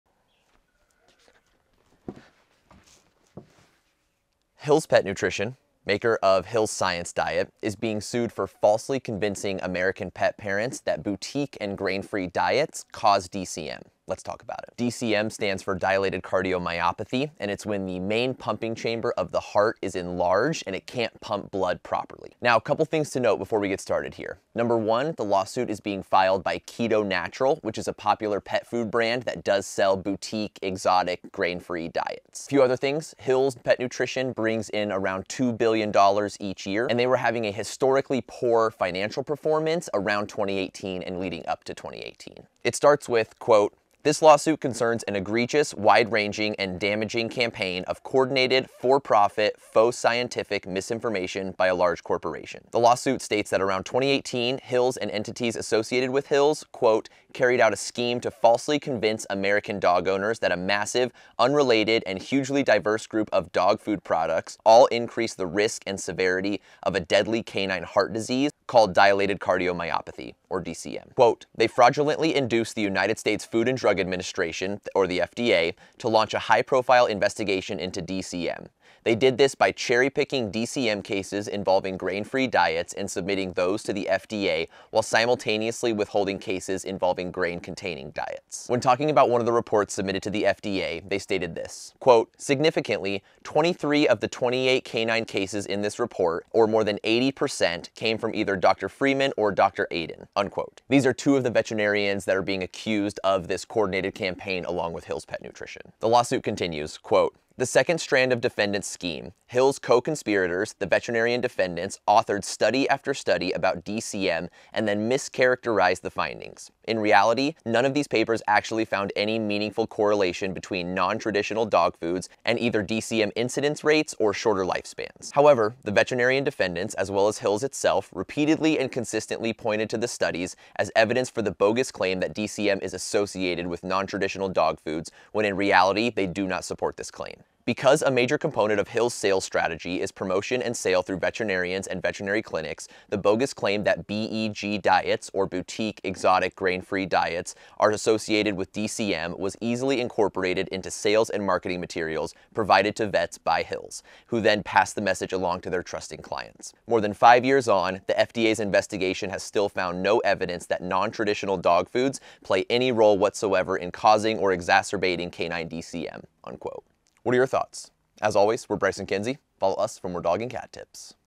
The $2.6B Hill's Lawsuit is INSANE!
In this video, we break down the details of the ongoing $2.6 billion class action lawsuit against Hill’s Pet Nutrition. The lawsuit, filed by KetoNatural Pet Foods, alleges that Hill’s manipulated the FDA’s investigation into grain-free dog foods and their connection to canine dilated cardiomyopathy (DCM). Hill’s and several veterinarians are accused of spreading misinformation to damage their grain-free competitors, causing significant market shifts. If you’re wondering, “What is Hill’s Science Diet being sued for?” or “Who is suing Hill’s Science Diet?”, this video has all the answers.